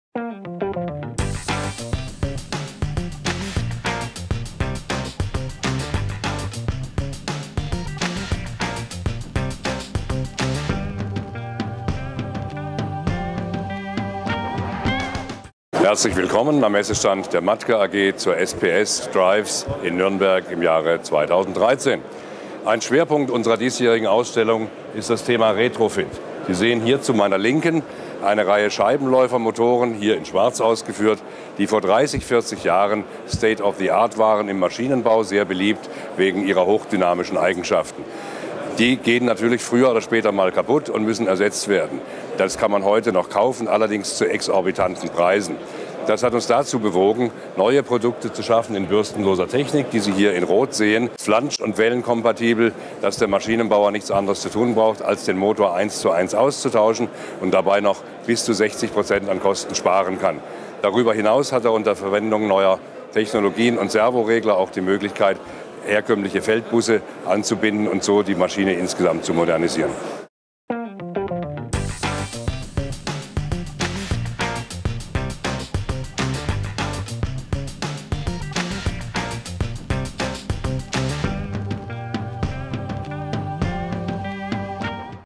Motoren für bessere Maschinen
Ein Schwerpunkt der Präsentation der Mattke AG auf der SPS IPC Drives war in diesem Jahr das Thema Retrofit. Am Beispiel von etwa 30 Jahre alten Scheibenläufermotoren demonstriert Mattke-Vorstand Werner Böhringer den Unterschied zwischen "alt" und "jung". "Neue Motoren sind bis zu 60 Prozent günstiger als der 1:1 Ersatz der in die Jahre gekommenen Motoren." Außerdem könne der Maschinenbauer bei dieser Gelegenheit durch die Anbindung moderner Regelungstechnik mit entsprechenden Feldbussystemen seine Maschinenperformance entscheidend verbessern.